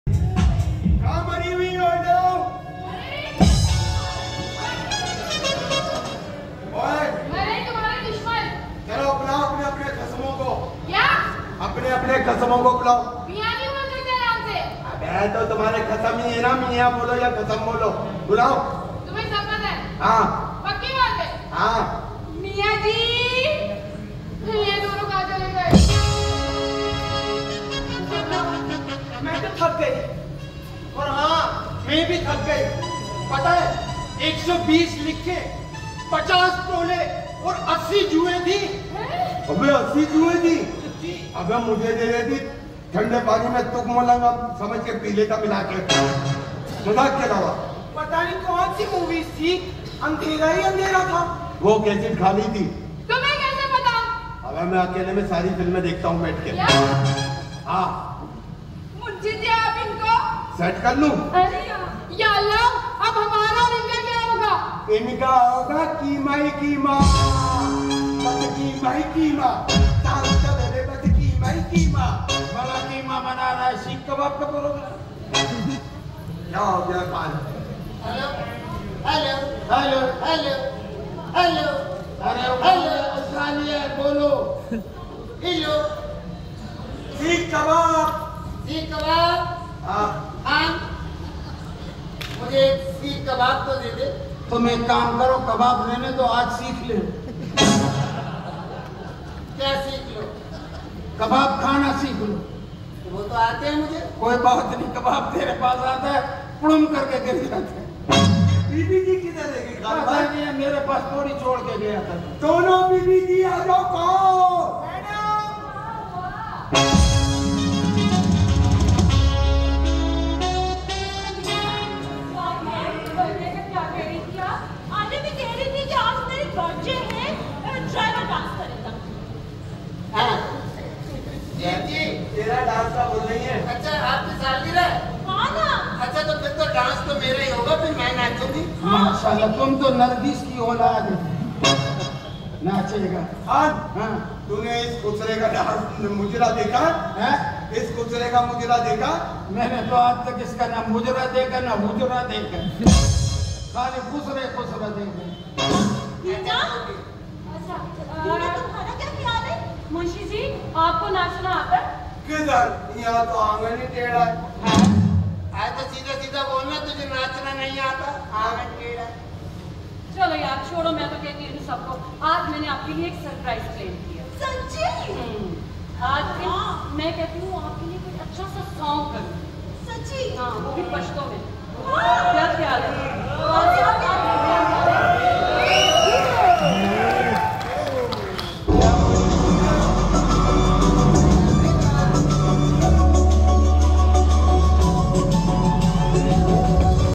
Part 21 Eid Jamalo Jamalo Comedy Stage Drama Rauf Lala, Wali Sheikh, Saba Sheikh ArtsCouncil Karachi
Part 21 Eid Jamalo Jamalo Comedy Stage Drama,
Comedian Rauf Lala, Comedian Wali Sheikh, Comedian Actress Saba Sheikh,  Arts Council Karachi,
Eid ul Azha Comedy stage drama 2024 Arts council karachi,